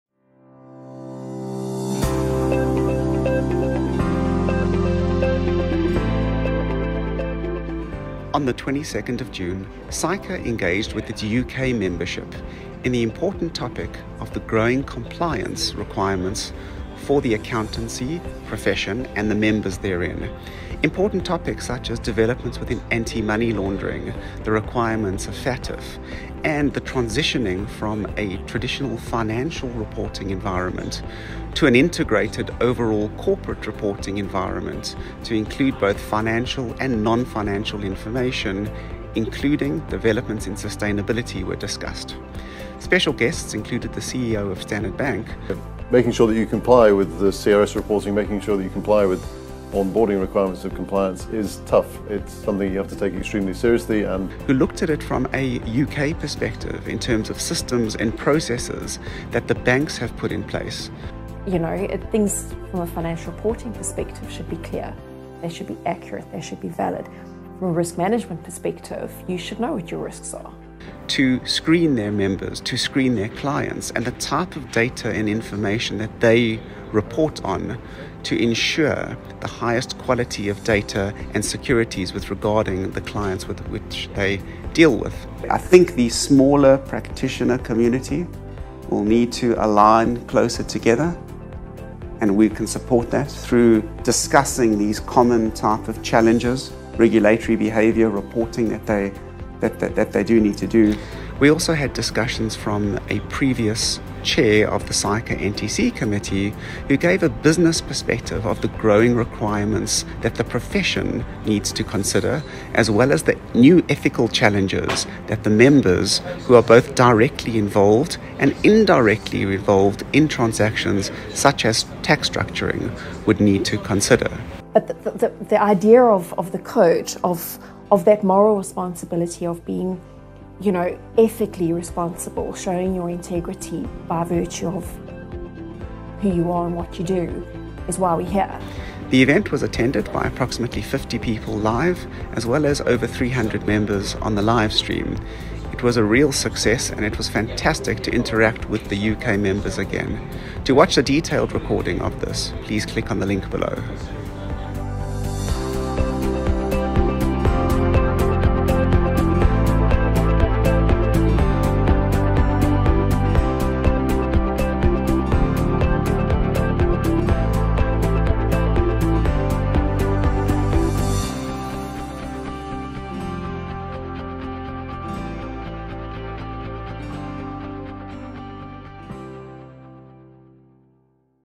Expanded Transactional Reporting highlights
Watch the Expanded transactional reporting highlights: the accountants burden discussion in London hosted by Standard Bank Global.